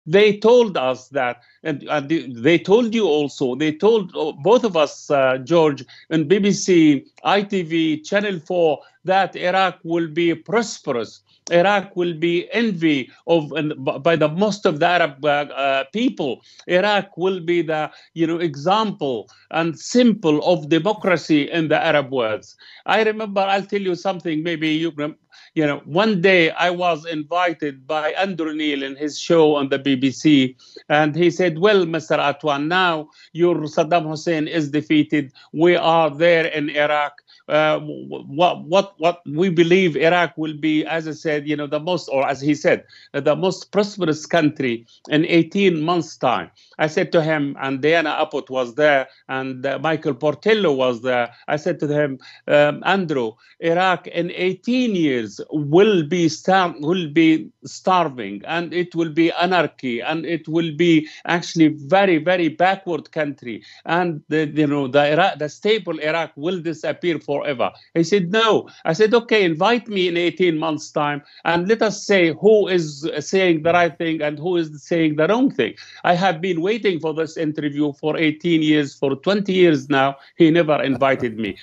The media were cheerleaders for the war on Iraq
"After the invasion I told Andrew Neil that in 18 years Iraq will be starving, recalls Abdel Bari Atwan. Invite me back in 18 months again. It’s 20 years now and he hasn’t invited me back

Follow MOATS on YouTube#AndrewNeil
BariAtwan"